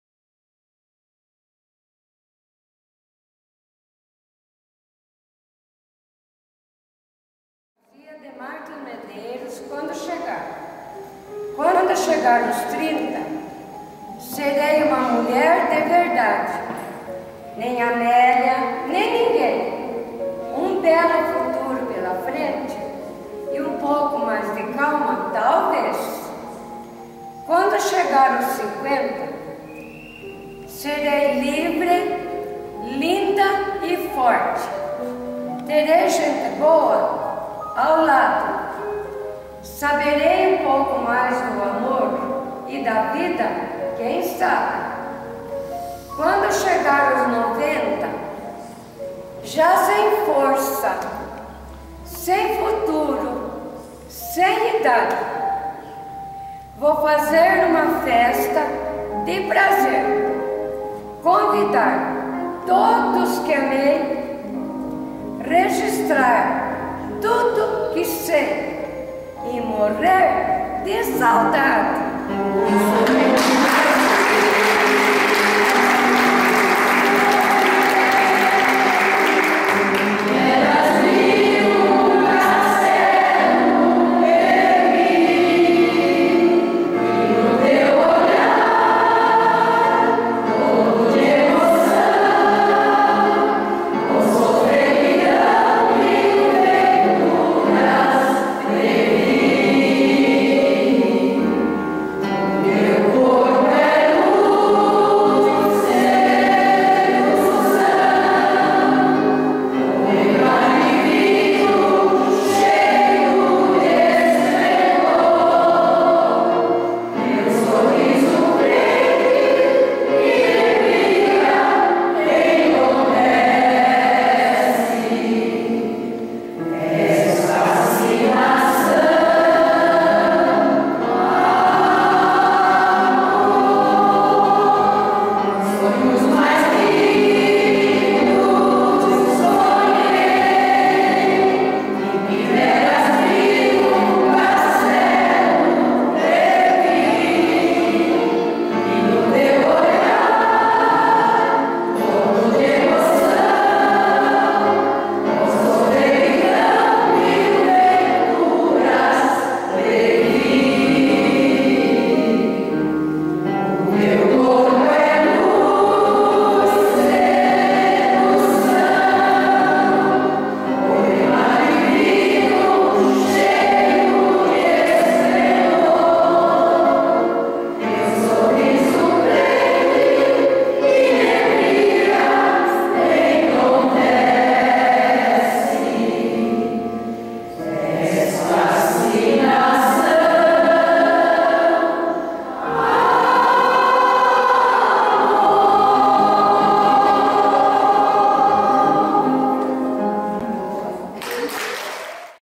22 02- Coral do Clube da Melhor Idade de Mairinque
Mairinque - SP - Brasil
Regência de Maria Candida Bertolini

Conheça e colabore com nossos projetos Compre um kit adesivos e nos ajude a construir o Pantheon de Minas! Divirta-se, emocione-se e inspire-se!!!